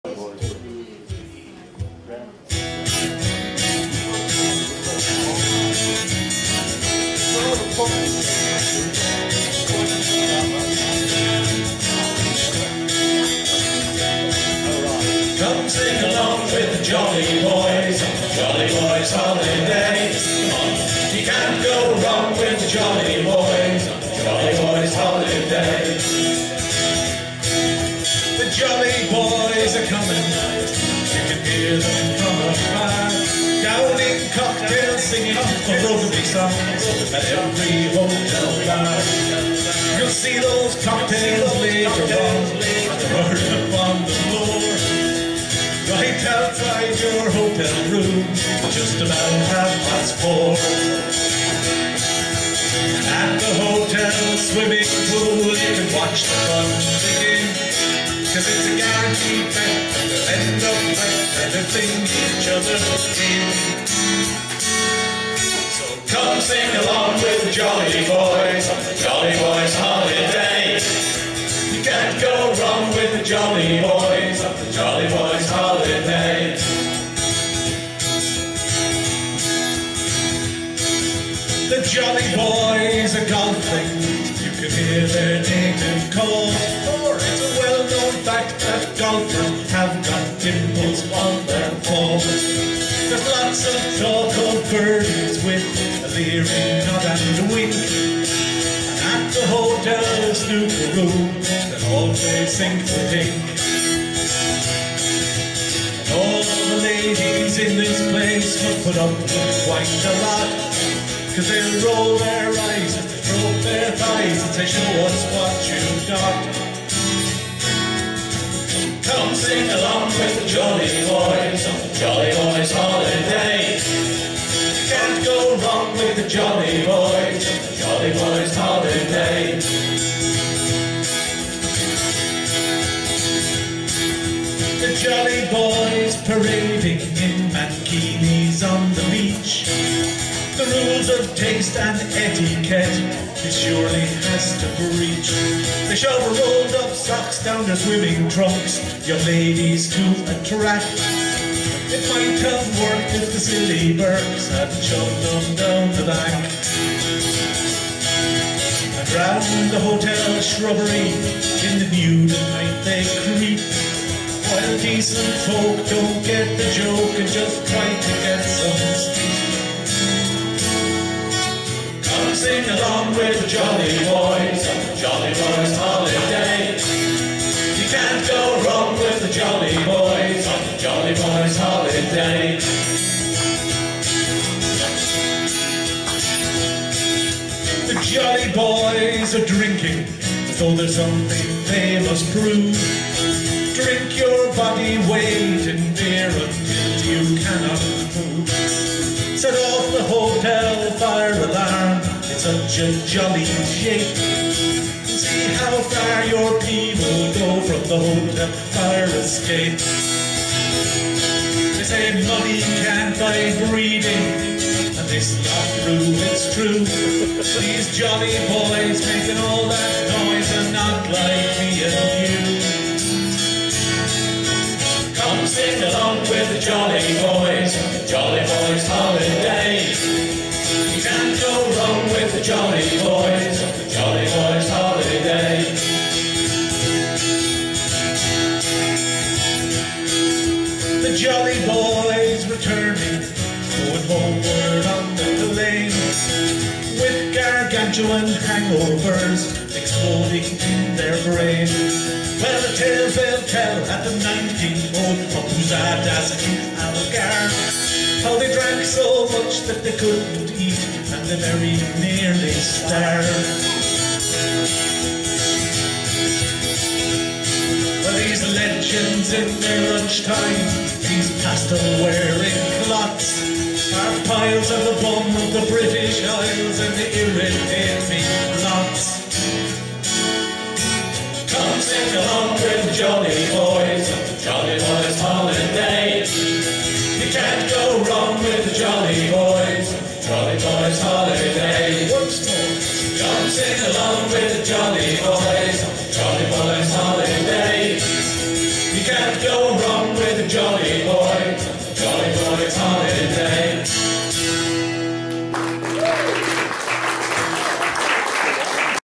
The Jolly Boys
An augmented & depleted NUNC (two additional musicians & no female vocalist) at Nuneaton Folk Club on 6 Feb 2019, videoed by John B. Smith